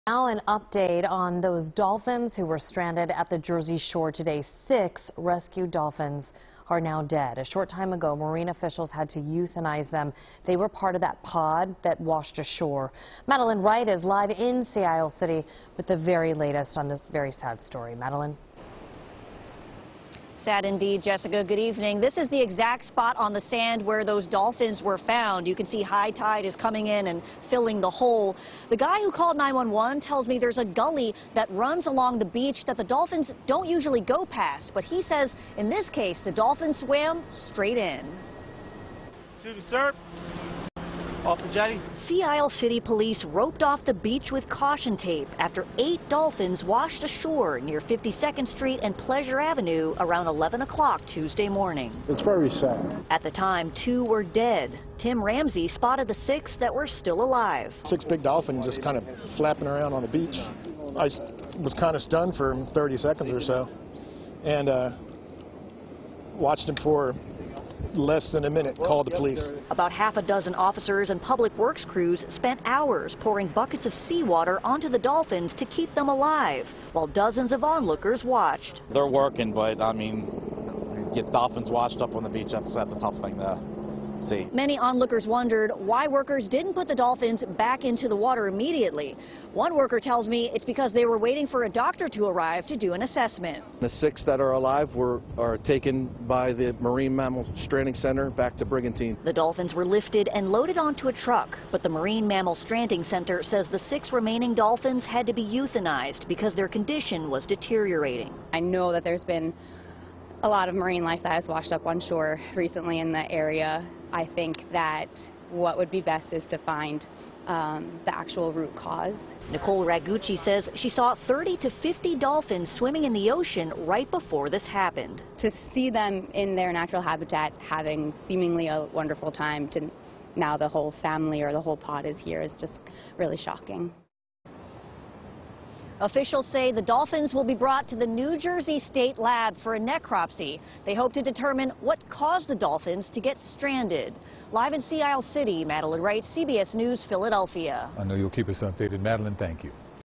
6 dolphins euthanized, 2 dead after washing ashore in Sea Isle City
Madeleine Wright reports.